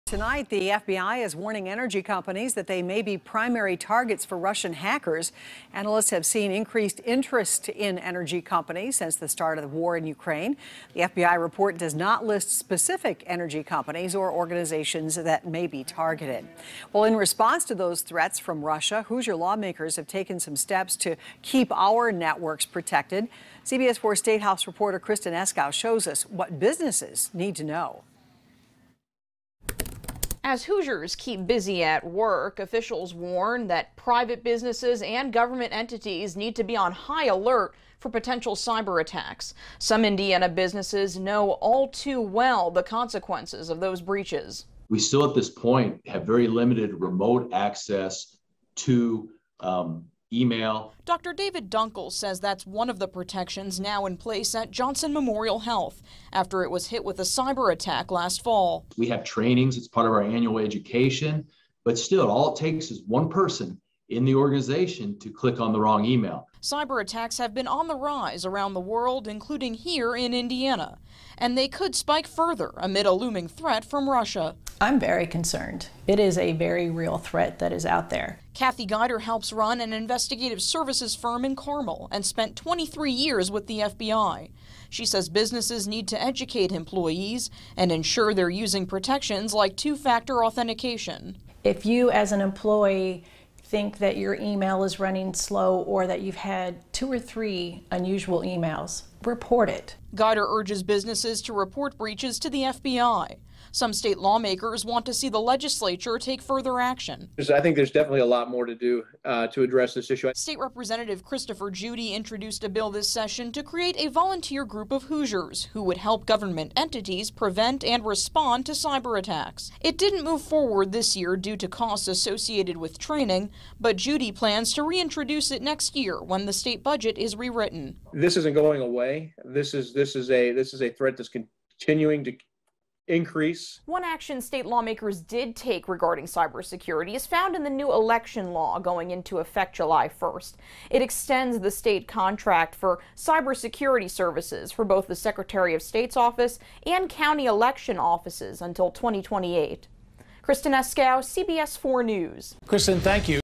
Indiana lawmaker to reintroduce cybersecurity bill next year
Some Indiana lawmakers say they want to see the legislature do more to try to stop cyberattacks, and one legislator plans to reintroduce a bill that died this past session.